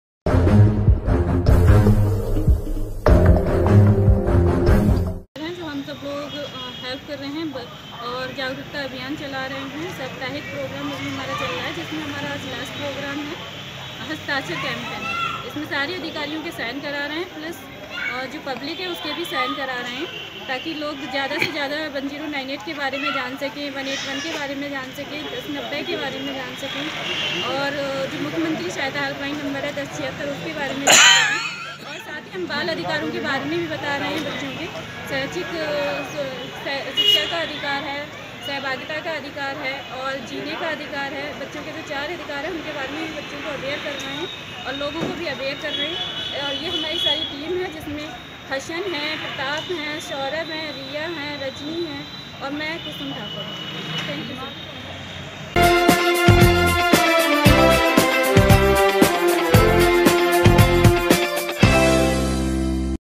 चाइल्डलाइन टीम द्वारा चलाया गया हस्ताक्षर अभियान | All Rights
आज दिनांक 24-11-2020 को चाइल्डलाइन टीम, बरेली द्वारा कलेक्ट्रेट, प्रोवेशन, महिला कल्याण, वरिष्ठ पुलिस अधीक्षक कार्यालय, मानव तस्करी यूनिट, उप श्रम आयुक्त कार्यालय, विकास भवन, बेसिक शिक्षा अधिकारी कार्यालय, डीआईओएस ऑफिस, बाल कल्याण समिति कार्यालय, एस पी सिटी कार्यालय और पुराना रोडवेज कार्यालय ए 0 आर 0 एम 0 कार्यालय  बरेली  में चाइल्ड लाइन से दोस्ती सप्ताह प्रोग्राम के अंतर्गत  सुरक्षा बंधन और हस्ताक्षर प्रोग्राम का आयोजन किया गया जिसमें बच्चो को मिशन शक्ति और सभी हेल्प लाइन नंबरों की जानकारी दी गई । जिसमें बालक और बालिकाओं को सभी हेल्प लाइन नंबरों जैसे 108 एंबुलेंस सेवा,  102 स्वास्थ्य सेवा, 1098 चाइल्ड हेल्प लाइन, 1090 वूमेन पावर लाइन, 1076 मुख्यमंत्री सहायता न0, 181 महिला हेल्प लाइन और 112 पुलिस हेल्प लाइन नंबरों के बारे में और मिशन शक्ति व महिलाओ के साथ हो रहें अपराधो के बारे में विस्तारपूर्वक बताया गया और बच्चो के बाल अधिकारों जैसे 1 शिक्षा का अधिकार, 2 जीने का अधिकार, 3 सहभागिता का अधिकार और 4  विकास का अधिकार के प्रीति भी बच्चो को जागरूक किया गया और बताया गया कि किसी बच्चे का बाल विवाह हो, बाल मजदूरी में कार्यरत कोई बच्चा दिखे या कोई ऐसा बच्चा आपको मिले जो अपने माता पिता और परिवार से अलग हो गया हो 1098 पर कॉल कर अवश्य सूचित करे ताकि वह बच्चा अपने परिवार तक पहुंच सके । चाइल्डलाइन 24/7 डे कार्य करने वाली एक मुफ्त आउटरीच सर्विस है। इस मौके पर चाइल्डलाइन केंद्र समन्वयक कुसुम ठाकुर और टीम मेंबर रिया, रजनी, इलमा, हसन, प्रताप, प्रीति, सरिता तिवारी और  सौरभ व समस्त कार्यालय अधिकारी और वहा उपस्थित कार्यालय स्टाफ उपस्थित  रहा ।।